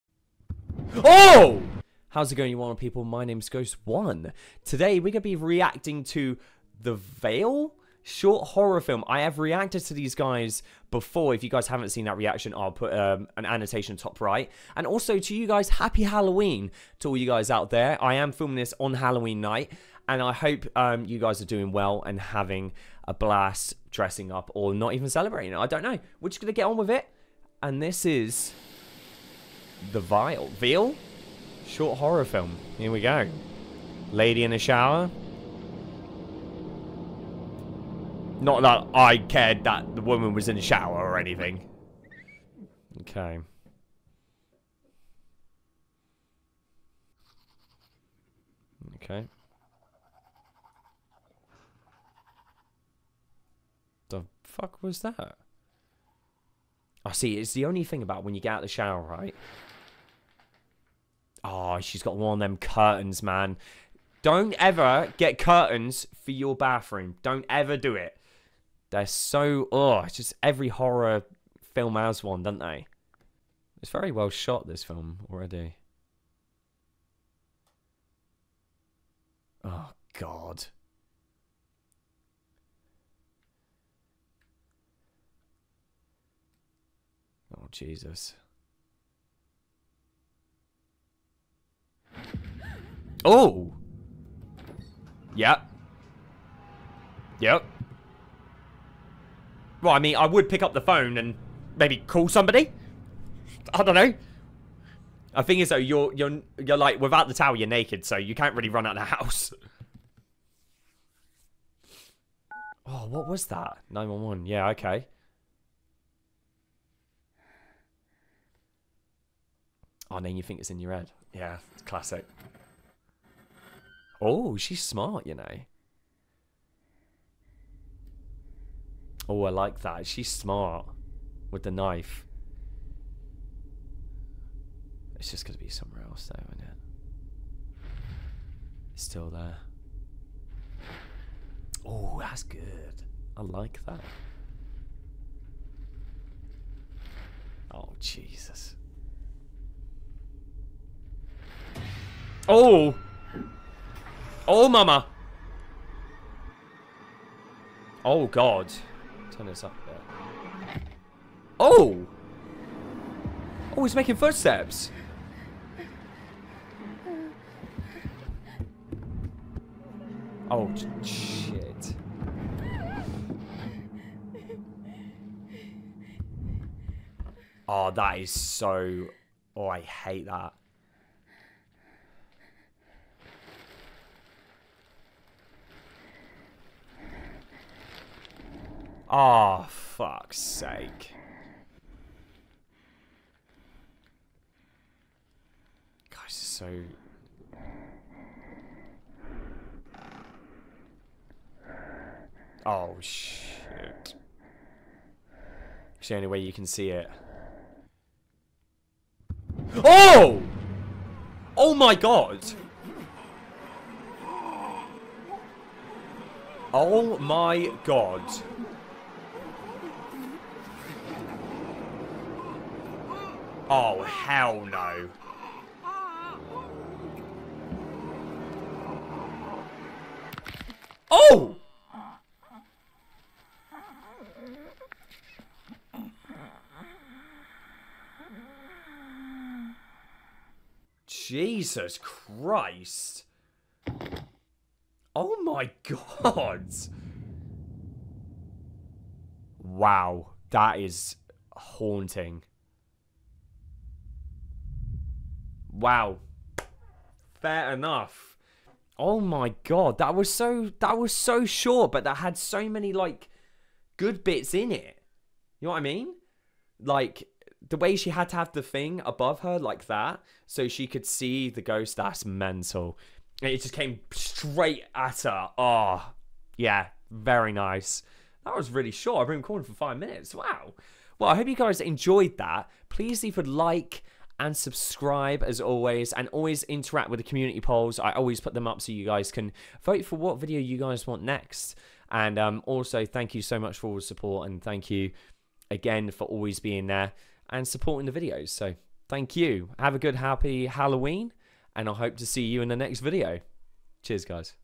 IT HIDES IN YOUR SHOWER?? A TERRIFYING short horror FILM! 😭 (reaction)
A reaction video to a short horror film hope you enjoy!